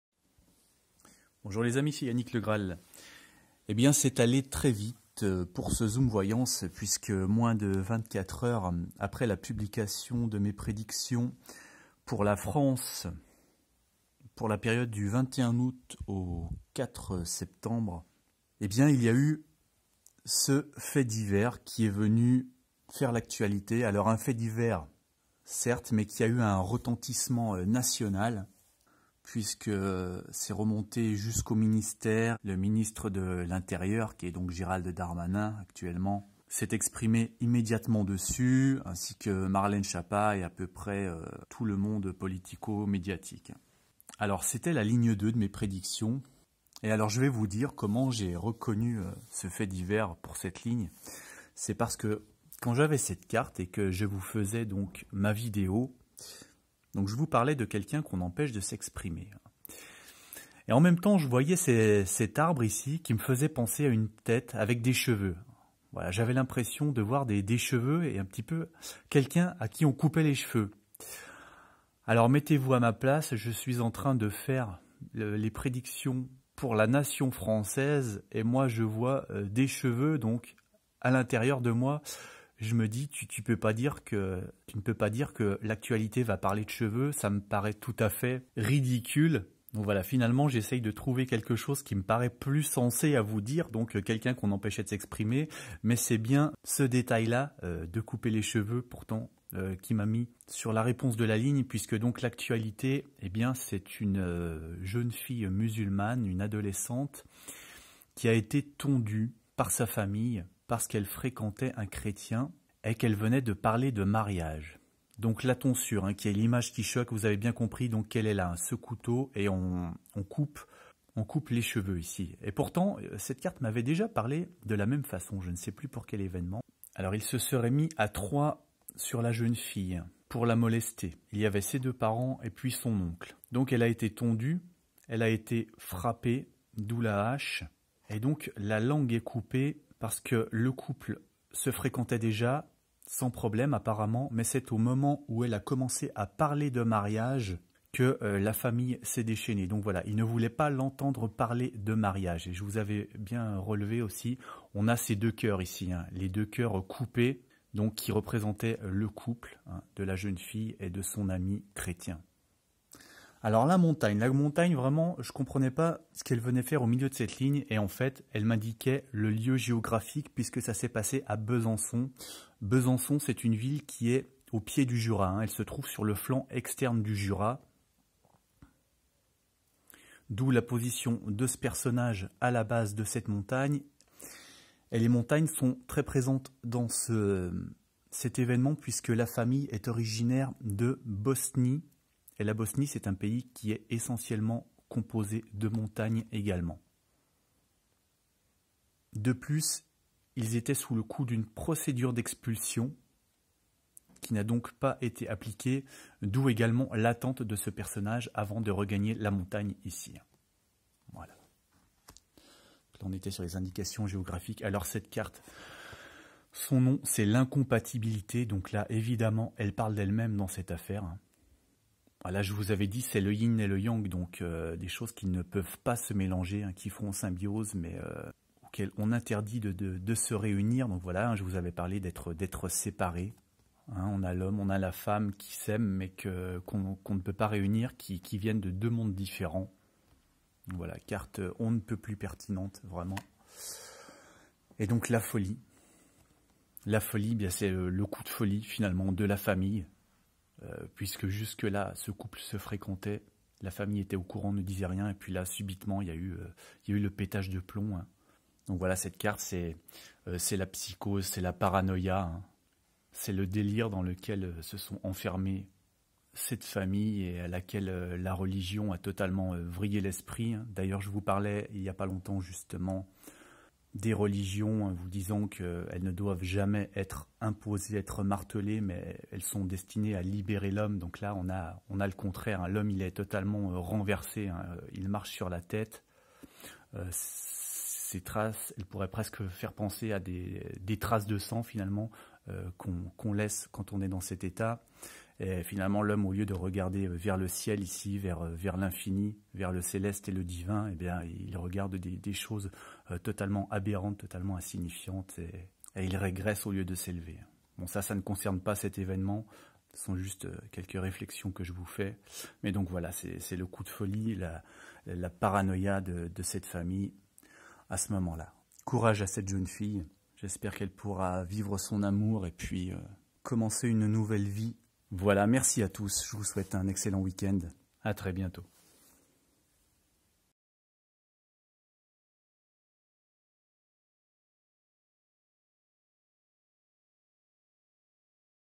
TONDUE POUR SON AMOUR - ZOOM VOYANCE
Emoi national après qu'une adolescente musulmane ait été tondue et battue par sa famille à cause de son amour pour un chrétien.